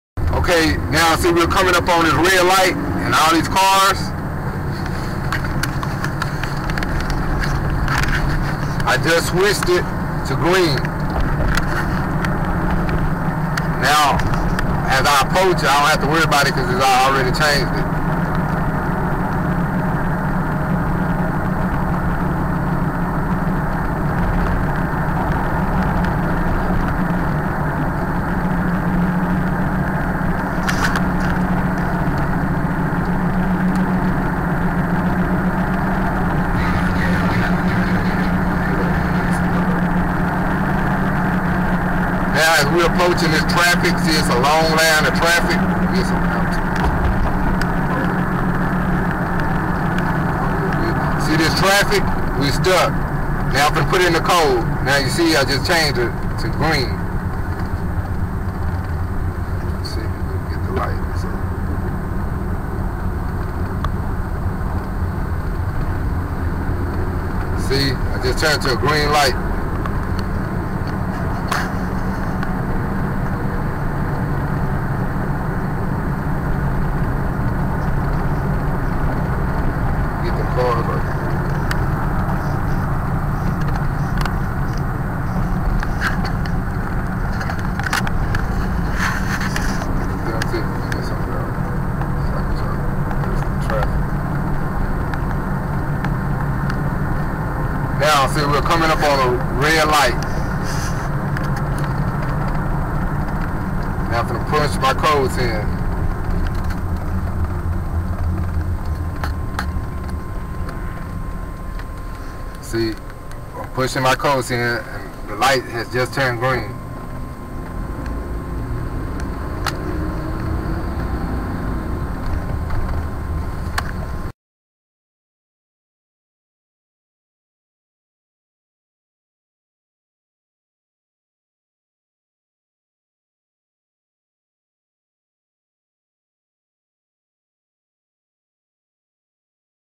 How to turn Red lights to Green lights with Cell Phone.
Another Broussard Brothers Production ~ How To Change Traffic Lights...Never be late to work again!